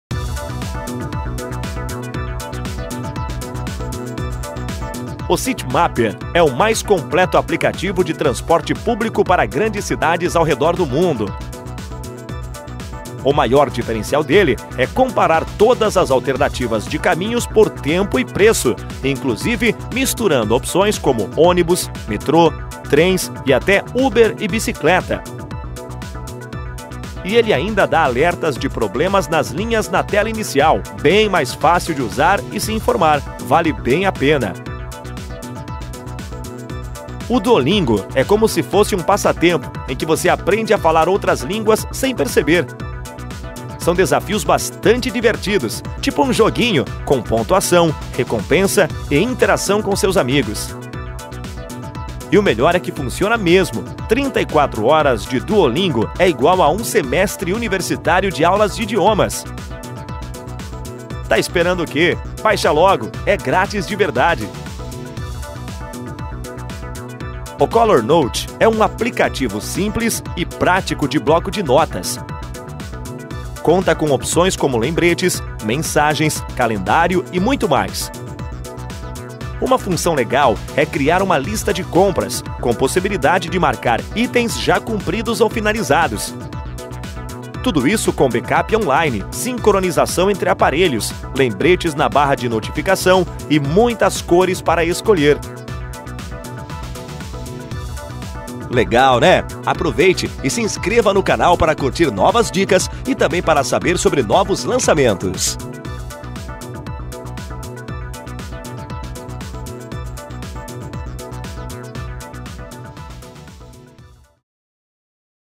3 Aplicativos Grátis Xperia Z5
O Citymapper é o mais completo aplicativo de transporte público para grandes cidades ao redor do mundo.
O maior diferencial dele é comparar todas as alternativas de caminhos por tempo e preço, inclusive misturando opções como ônibus, metrô, trens, e até uber e bicicleta.
E ele ainda dá alertas de problemas nas linhas na tela inicial. Bem mais fácil de usar e se informar. Vale bem a pena.

O Color Note é um aplicativo simples e prático de bloco de notas. Conta com opções como lembretes, mensagens, calendário e muito mais.
Uma função legal é criar uma lista de compras, com possibilidade de marcar itens já cumpridos ou finalizados.
Tudo isso com backup online, com sincronização entre aparelhos, lembretes na barra de notificação, e muitas cores pra escolher.

O Duolingo é como se fosse um passatempo em que você aprende a falar outras línguas sem perceber.
São desafios bastante divertidos, tipo um joguinho, com pontuação, recompensa e interação com seus amigos.
E o melhor é que funciona mesmo: 34 horas de Duolingo é igual a 1 semestre universitário de aulas de idiomas. Tá esperando o quê? Baixa logo, é grátis de verdade.